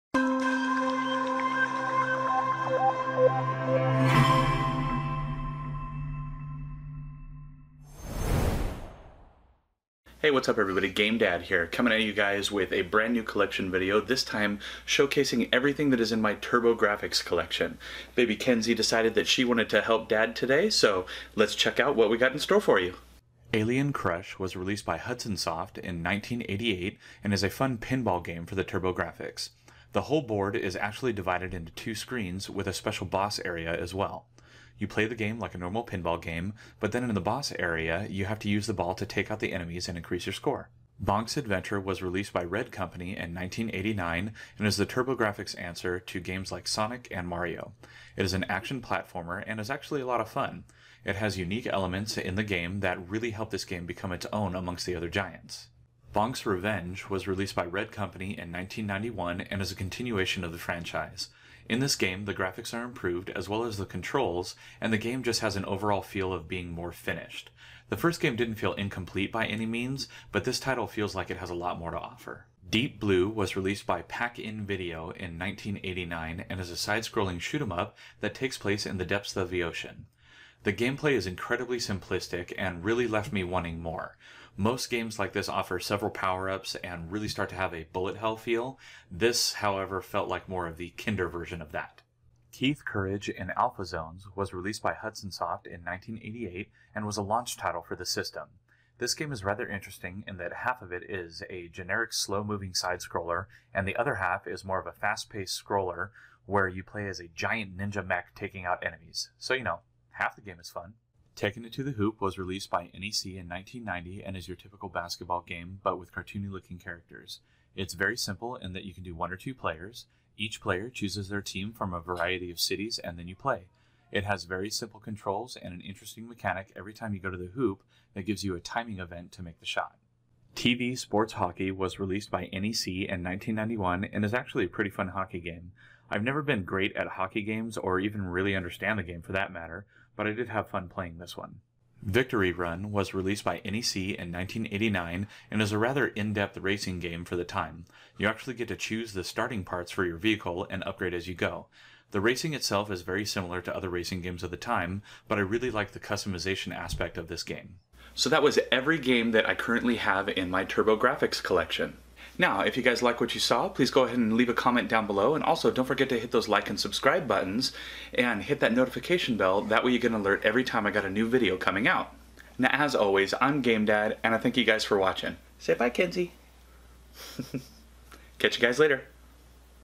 TurbGrafx 16 Collection | GameDad
I hope you enjoyed this look at my TurboGrafx Collection. I've had a lot of fun collecting for this console and can't wait to add even more! Any must have games I should know about?

Let me know what you thought this video down below!

For Mail: TBA